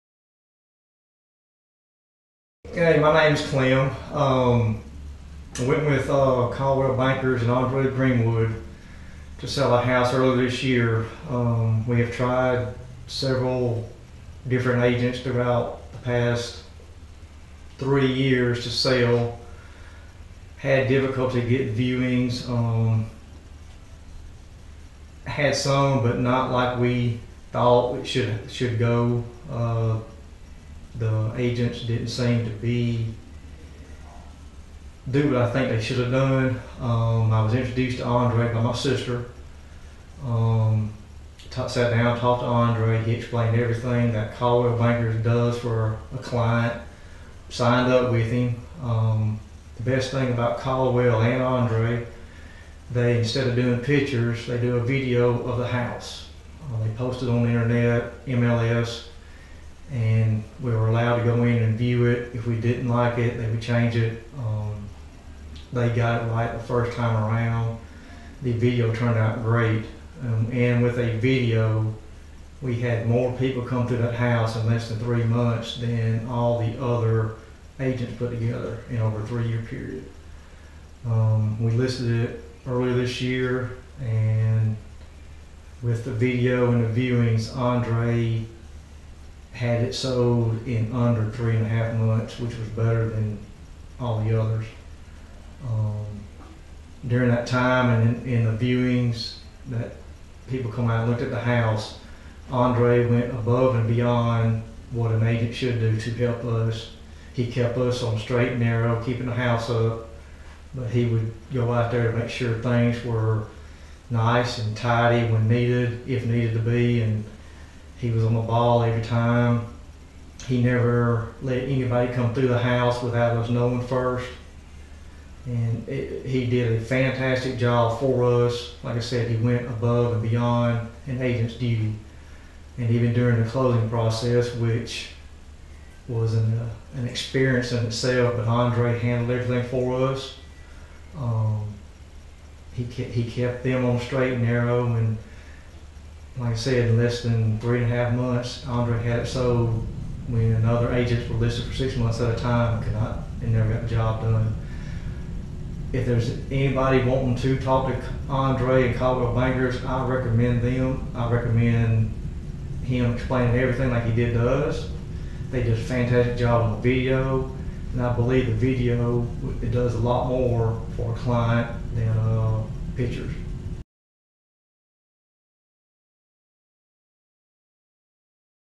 Testimony from Clem, a satisfied Seller
Clem had a beautiful 3 bedroom 2 bath home set in a countryside neighborhood. I helped him sell it in 3.5 months.